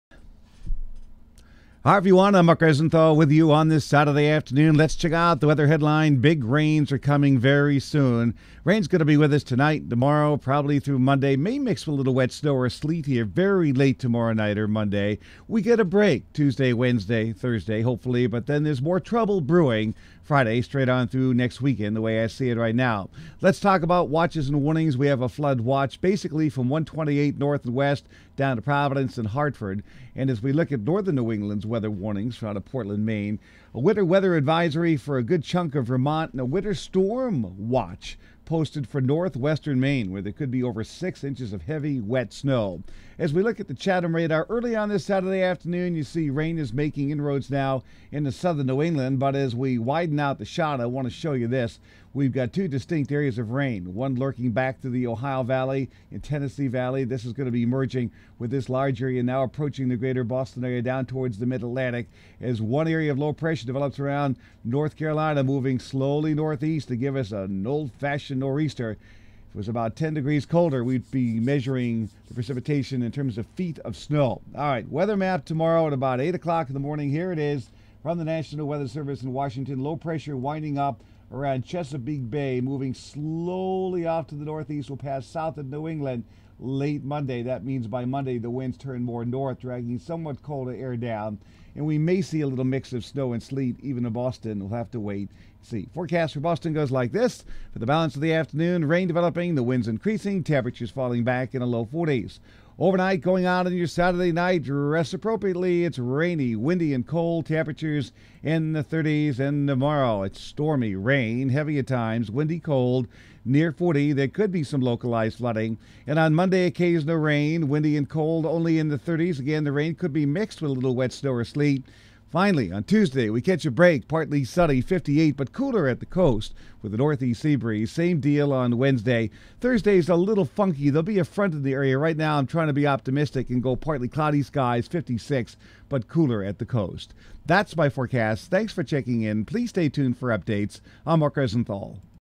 Weatherblast Forecast Updated at 1:57pm on 3/29/14
Becoming rainy windy 40s.